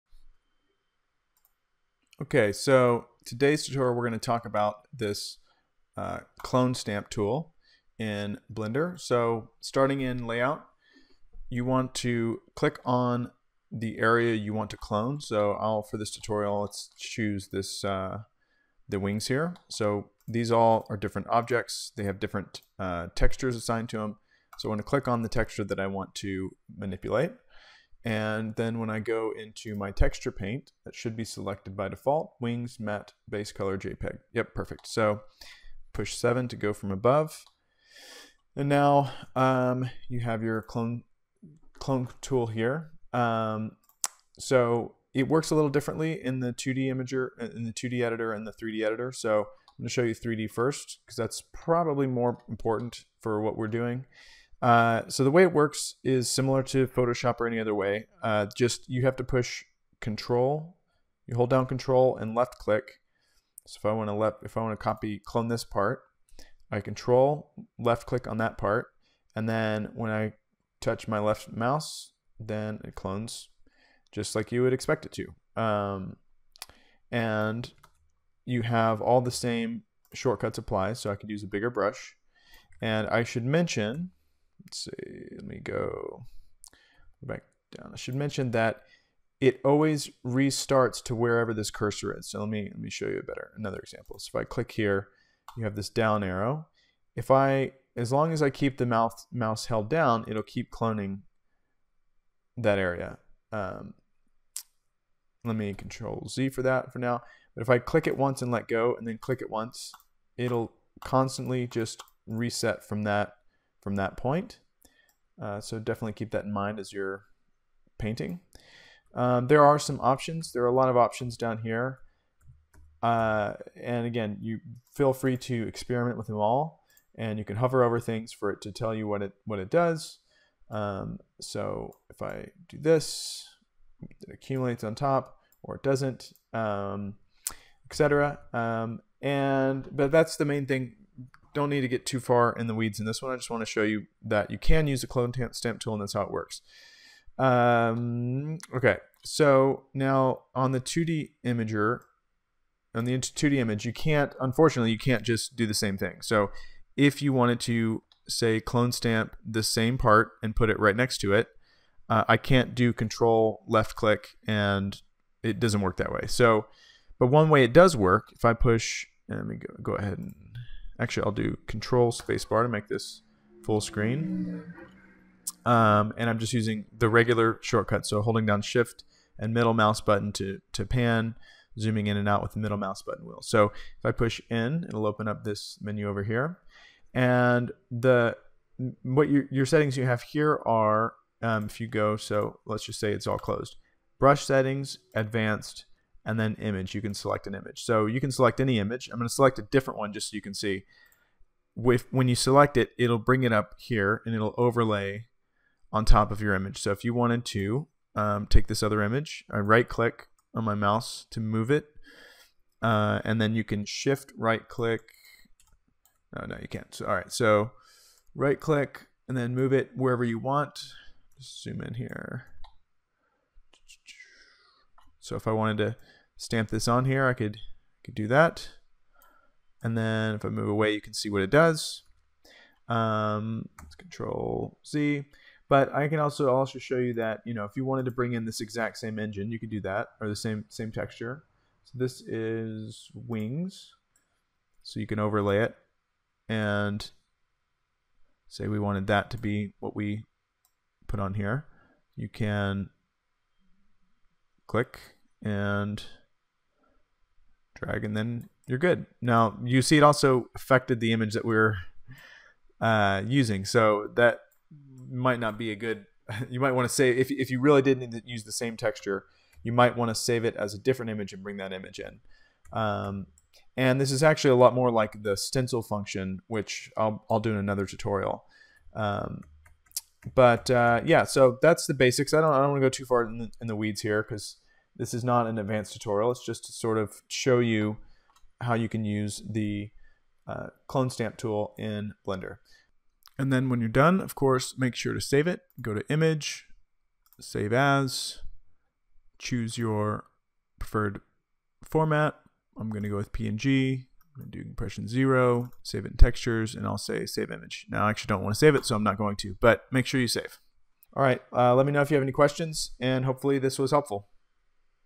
Clone Stamp Tool in Blender Tutorial!
In this video, I'll show you the basics of how to use the clone stamp tool in Blender.

00:00 - Intro
00:05 - Getting Started
00:54 - Stamp tool in 3d editor
02:55 - Stamp tool in 2d image editor
04:53 - SAVE YOUR IMAGE!
05:11 - Outro